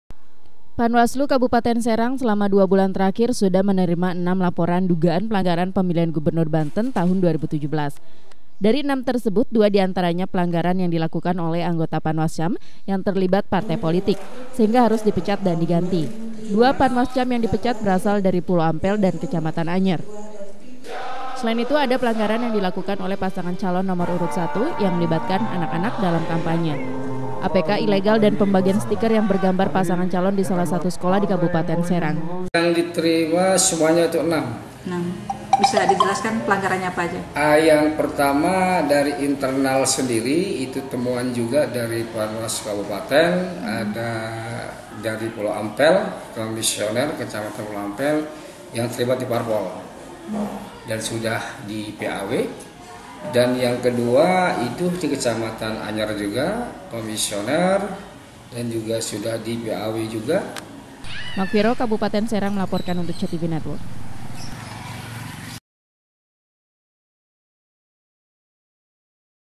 Panwas Terima 6 Laporan Pelanggaran Pilkada
Panitia Pengawas Pemilu Kabupaten Serang telah menerima sebanyak enam laporan dugaan pelanggaran Pemilihan Umum Kepala Daerah. Dari 6 laporan tersebut, dua diantaranya pelanggaran yang di lakukan oleh Panwascam  karena terlibat dengan Parpol. (Magfiroh)